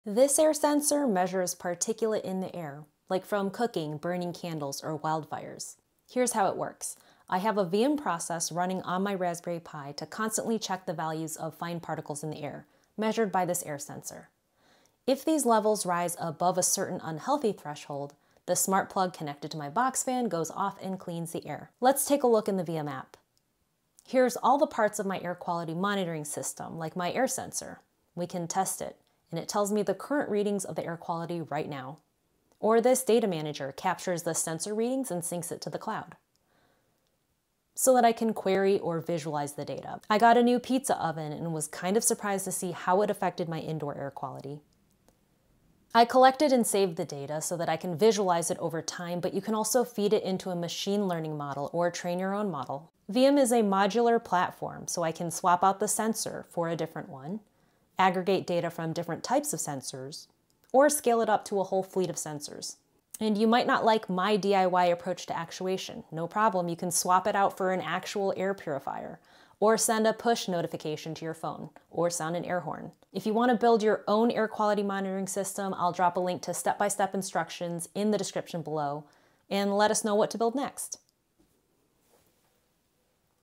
Air quality monitoring | Demo
Leaving an air filtration system running at all times is inefficient but you also don't want to forget to turn it on if the air quality reaches concerning levels. Use Viam to automate a Raspberry Pi air filtration system using air quality sensors and an air filter attached to a box fan. Set up a conditional workflow to turn on the air filtration when particulate values rise above a specified threshold, and then turn it off again when the air is clean.